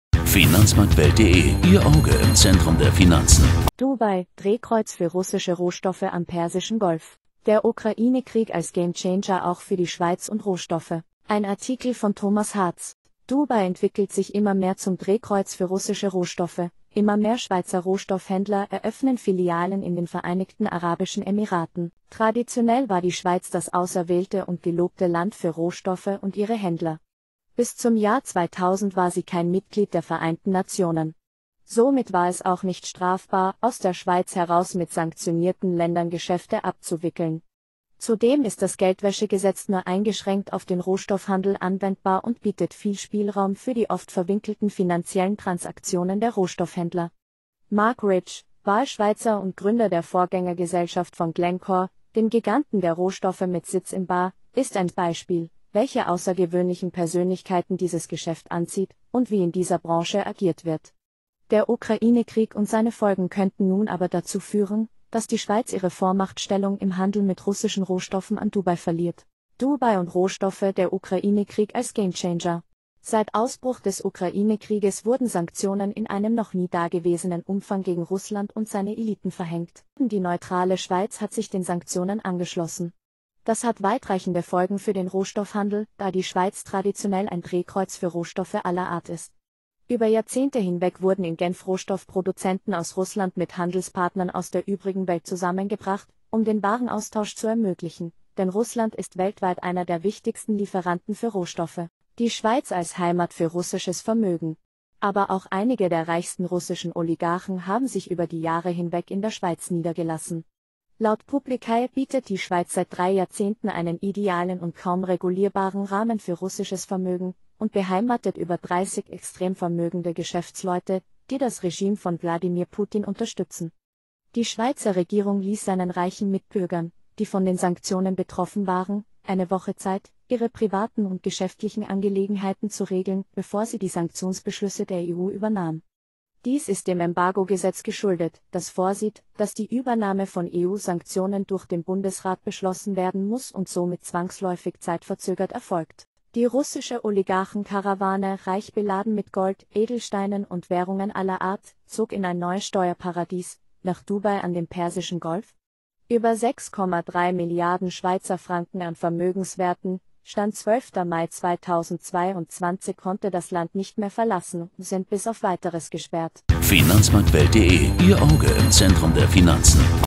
Dubai – Drehkreuz für russische Rohstoffe am Persischen Golf.
Der Ukraine-Krieg als Game Changer auch für die Schweiz und Rohstoffe. Dubai entwickelt sich immer mehr zum Drehkreuz für russische Rohstoffe – immer mehr Schweizer Rohstoffhändler eröffnen Filialen in den Vereinigten Arabischen Emiraten. Traditionell war die Schweiz das auserwählte und gelobte Land für Rohstoffe und ihre Händler - aber das ist nun wohl vorbei! Das neue Zentrum für den Rohstoff-Handel wird Dubai..
Musik, Bilder und Videos stammen von CyberLink Corporation /PowerDirector